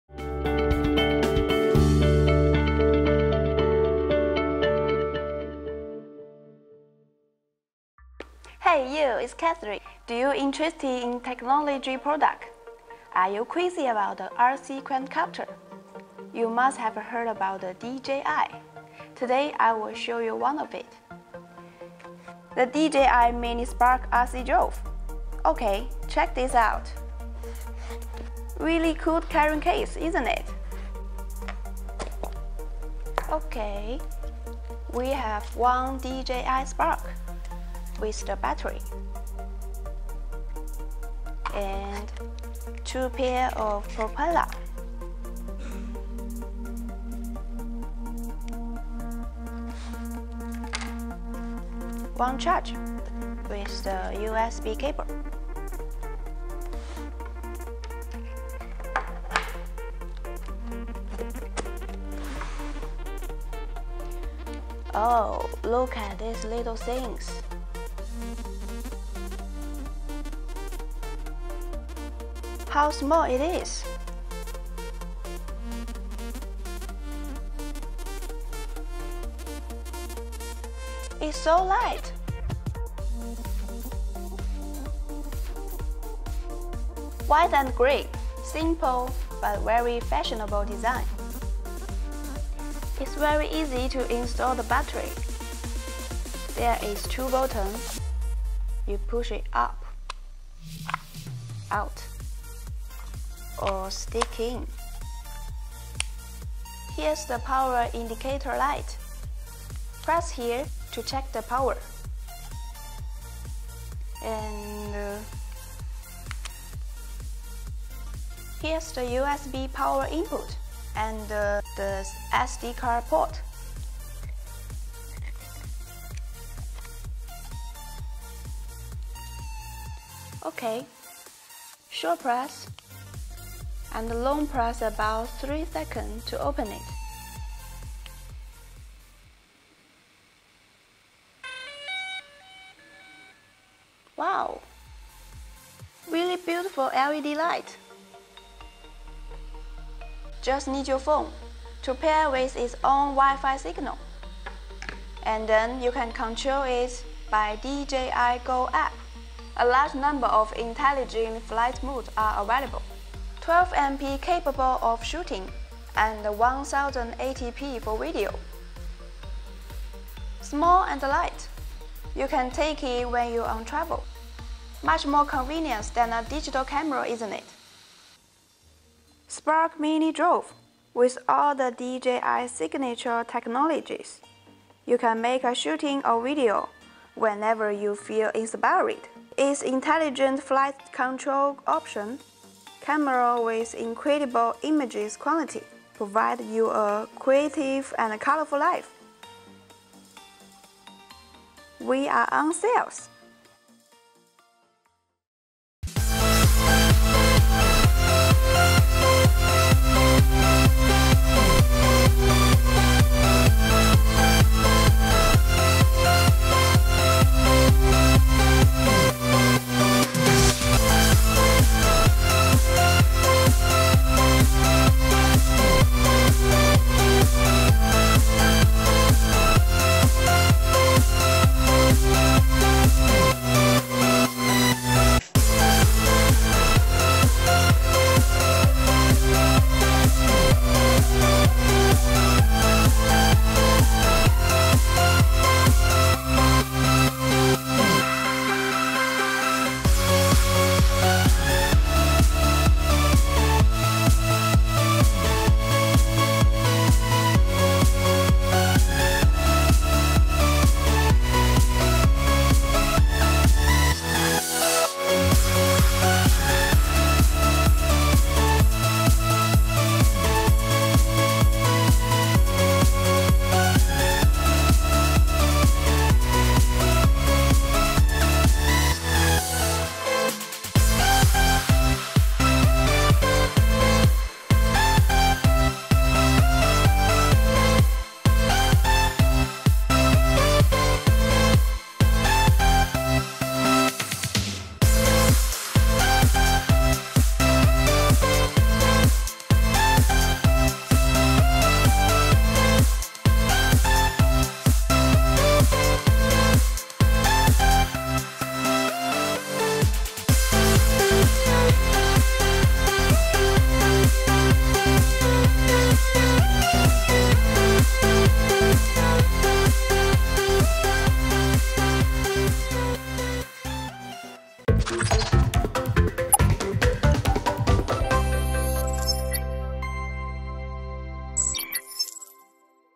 DJI Spark Mini RC Selfie Drone - BNF
Features:
Maximum flight time of 16mins
Maximum speed of 50km/h
Camera with 1/2.3in sensor 12MP capable of shooting 1080P video
Three TapFly modes, TapFly forward; TapFly backward; TapFly free
Forward and downward of obstacle avoidance
Using the DJI GO app, a large number of intelligent flight modes are available
Automatically recognizes subjects, follows and captures them as they move, making it easier to get complex shots